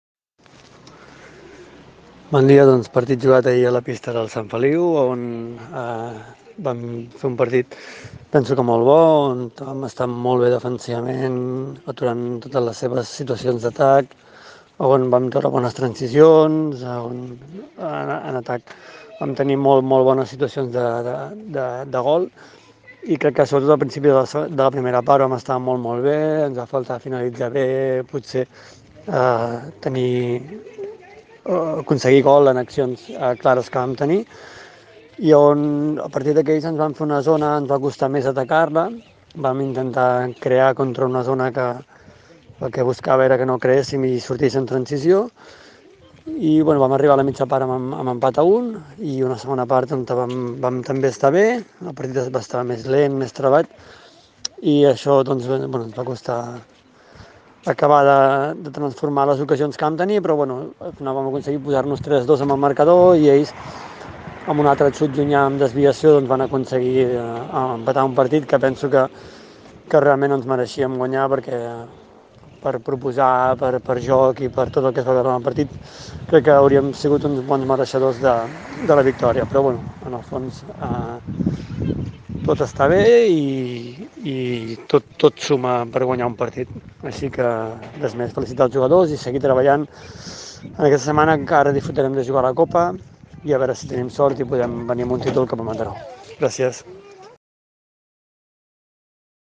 🗣🎙 Declaraciones de Albert Larrea (Entrenador CH Mataró Masculi)tras la jornada 19 lliga 🏑 okligapla
🟦⬜ Declaraciones de Albert Larrea (Entrenador CH Mataró Masculi)tras la jornada 19 lliga 🏑